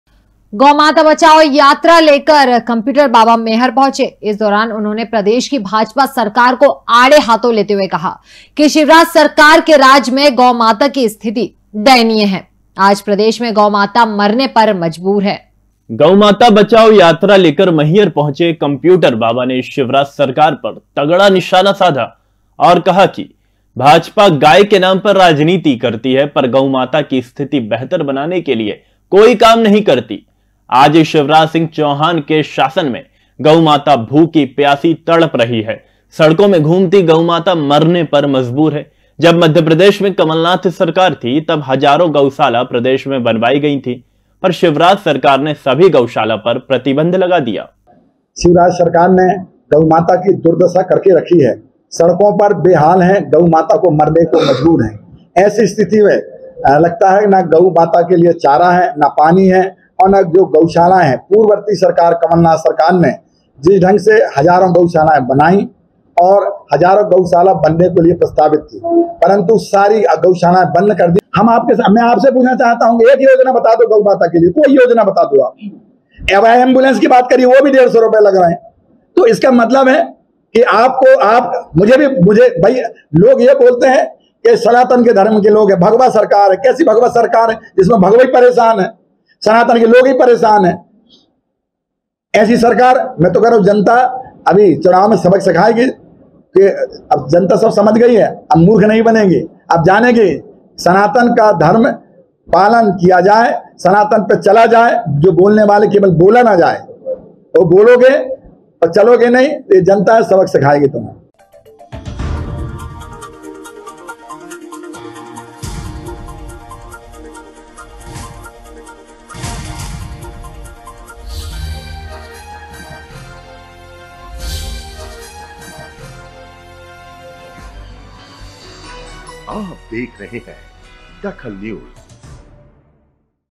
DAKHAL NEWS मैहर पहुंची गौ माता बचाओ यात्रा
मैहर पहुंची गौ माता बचाओ यात्रा
कंप्यूटर बाबा ने शिवराज पर साधा निशाना